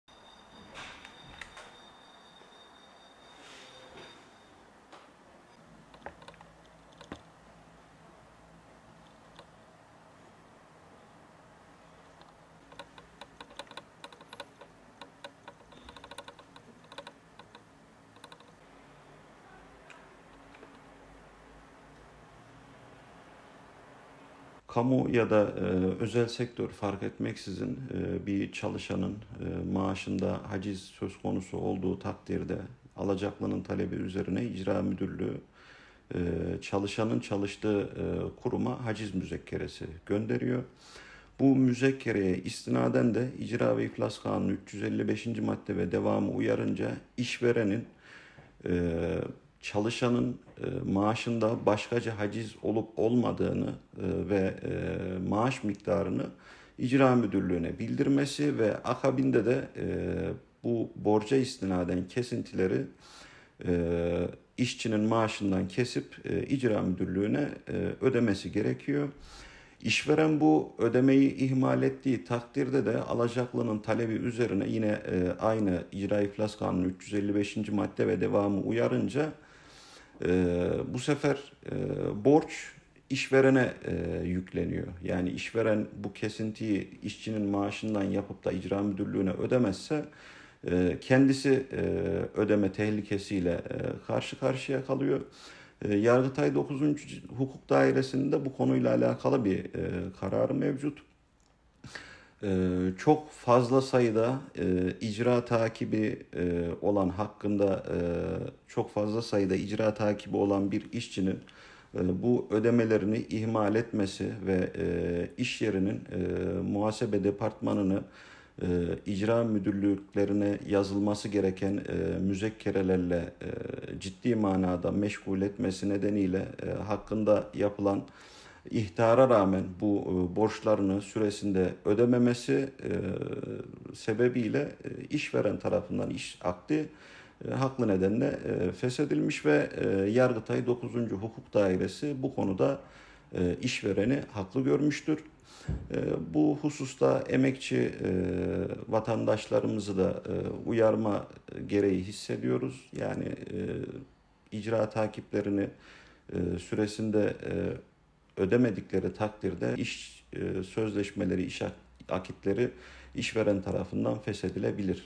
Fazla sayıda haciz, iş akdini fesih edebilir
Avukat Seyit Yağmur, çalışan işçinin maaşı üzerindeki çok sayıda haciz işleminin işletme muhasebesindeki iş yükünü arttırması nedeni ile işveren tarafından iş akdinin haklı olarak fesihe sebep olabileceğini söyledi.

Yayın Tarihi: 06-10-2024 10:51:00